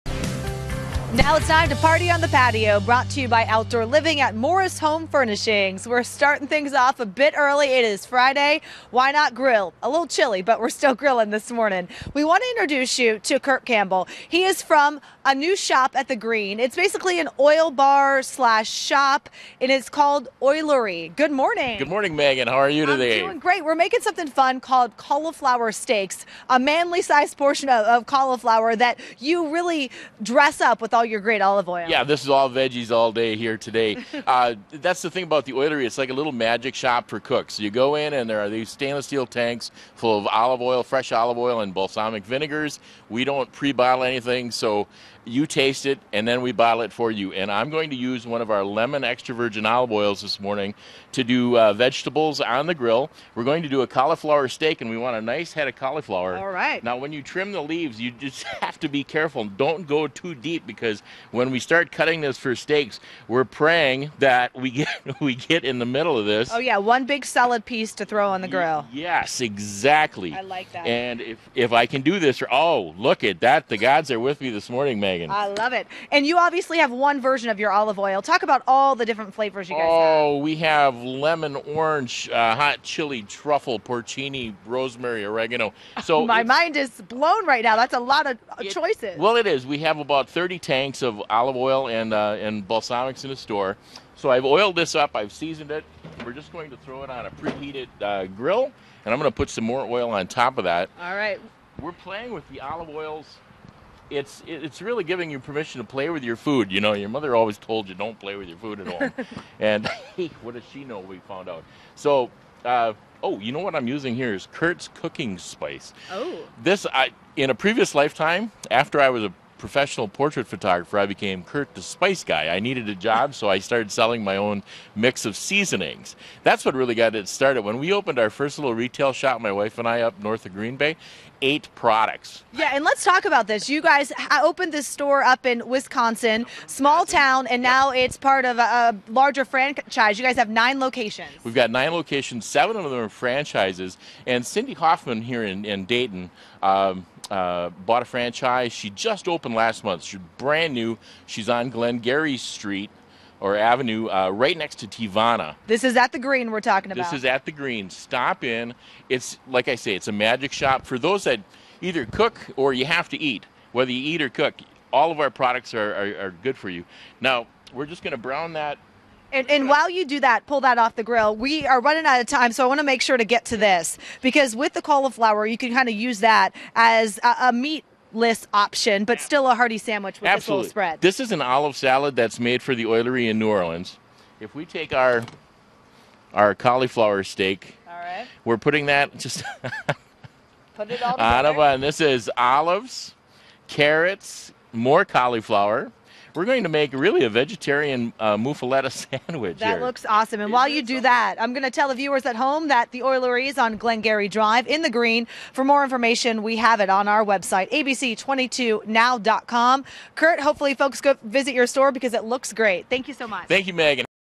The Oilerie @ The Greene
Recipe for Grilled Cauliflower Steak

Ingredients
1 large head of farm fresh Cauliflower
Curt's Cooking Spice to taste , available at The Oilerie Dayton
1/4 C  Oilerie Lemon extra virgin olive oil divided

Method

Pour some olive oil on a paper towel and oil the grill rack. Heat grill to medium high, about 425 degrees.
Gently remove the leaves and cut the stem from the head of cauliflower leaving the base of the stem slightly protruding from the head, about 1/8". Gently wash and pat dry.
Place the cauliflower on a sturdy cutting surface stem side down.  Beginning on either side, cut 1/4 of the head away with a large knife. Reserve the end pieces. From the middle of the head cut
two or three very thick slices, 1 1/4" to 1 1/2" wide. Gently lay each "steak" on a baking sheet. You are trying to keep each steak intact.  Spread the end pieces, which have politely fallen somewhat apart
On the baking sheet as well. Drizzle half of the olive oil over the cauliflower and season with the Cooking Spice or salt